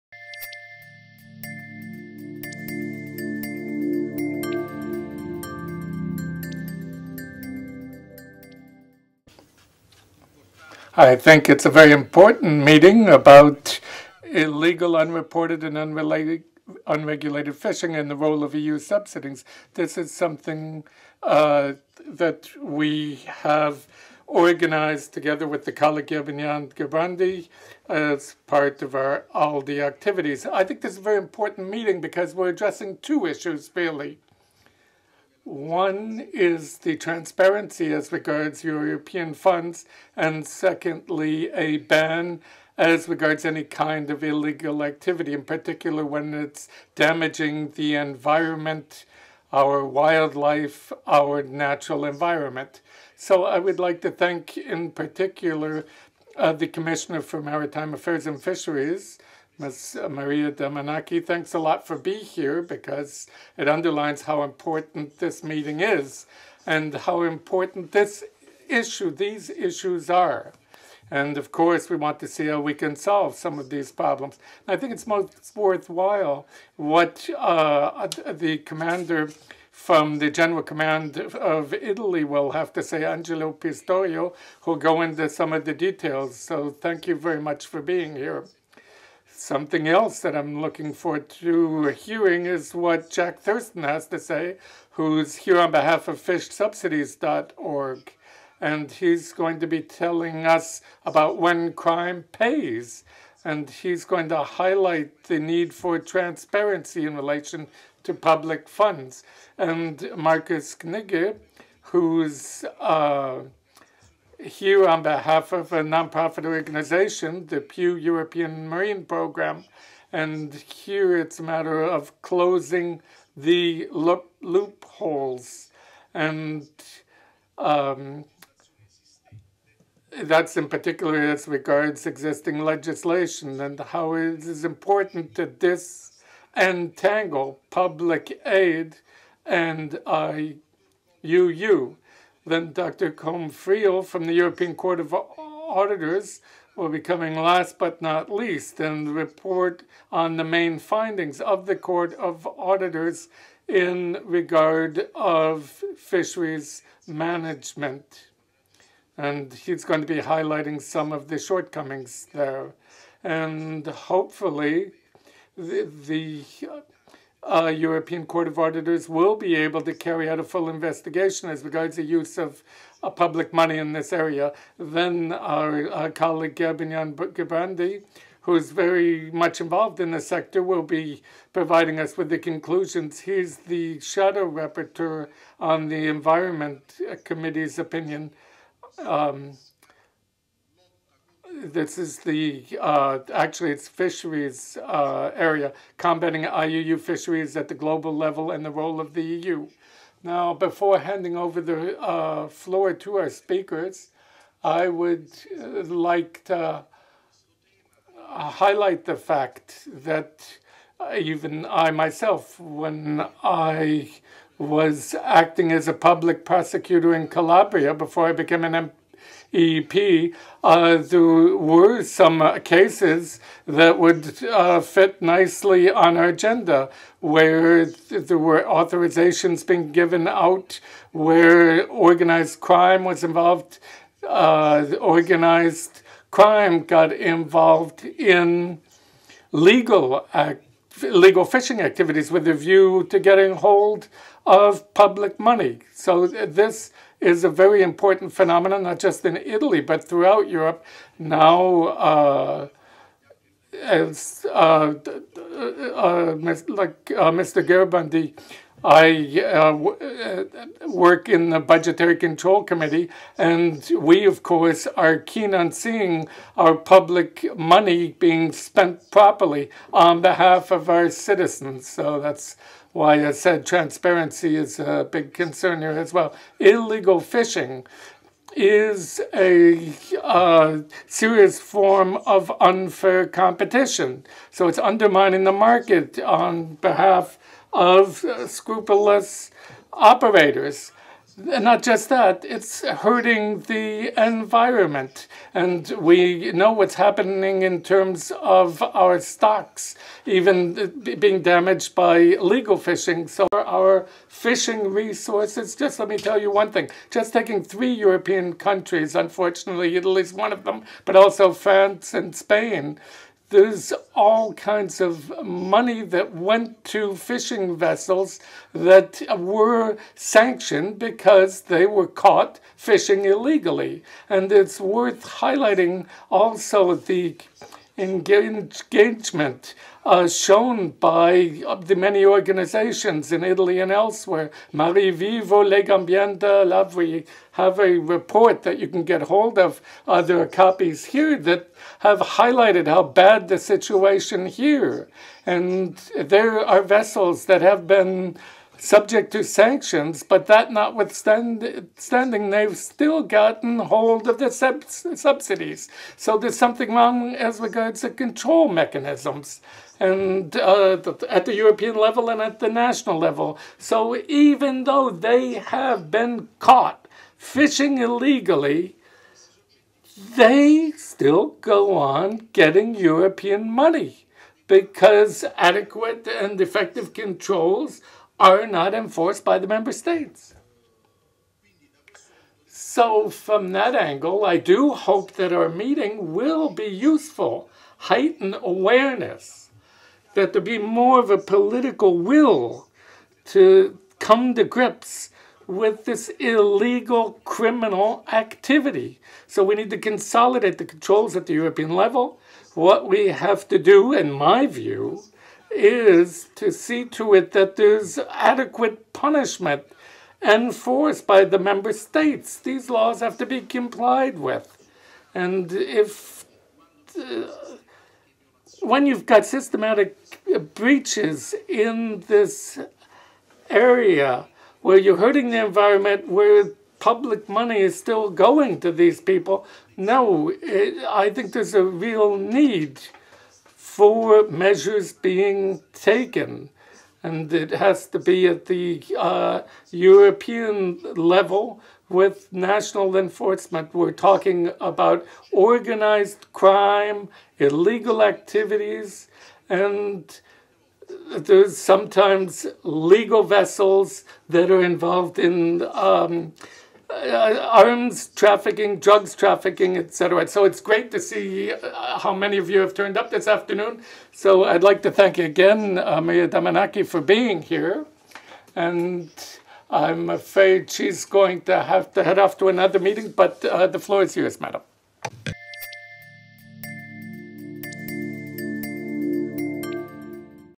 Luigi de MAGISTRIS - Illegal Fishing in the European Union [MEP Speech in Event] [MESP] [EN]
Speech in Event by MEP Luigi de MAGISTRIS on: Illegal Fishing in the European Union

Event date: Wednesday 23 March 2011 16:30 to 19:00
Location: BE BRUSSELS P5B001

The seminar will provide an opportunity to inform on subsidized vessels engaged in IUU fishing, address the current loopholes in the current structural policy, identify measures that would ensure that public aid supports the recovery of fish stocks, facilitates the transition towards sustainable fisheries and provides value to society.

[ALDEADLE-EVENT] [MEP-SPEECH] [SPEECH-IN-EVENT] [SPEV] [VIDEO] [EVENT] [HEARING] [Language EN]

110323-ALDEADLE-VIDEO-MEP-SPEECH-IN-EVENT-SEMINAR-De_Magistris-on-Illegal_Fishing